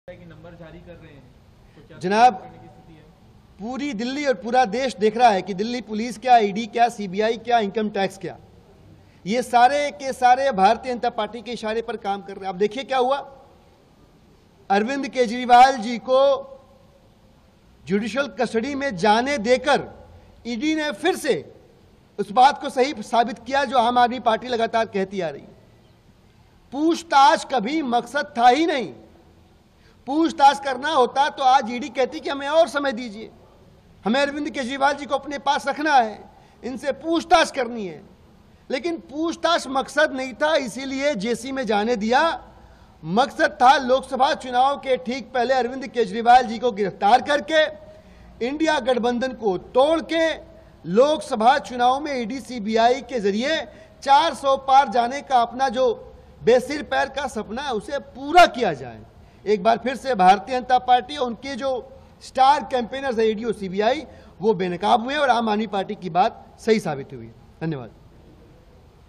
AAP's Dilip Pandey On Delhi CM Kejriwal Sent To Judicial Custody Till Apr 15: Purpose Was To...
On Delhi CM Arvind Kejriwal sent to judicial custody till April 15, AAP leader Dilip Pandey says, "The entire country is watching how Delhi Police, ED, CBI, and IT are all working on the instructions of BJP. Arvind Kejriwal has been sent to judicial custody and it was not opposed by ED, it shows that interrogation was not the purpose. The purpose was to arrest Arvind Kejriwal just before the Lok Sabha elections and break the INDIA alliance..."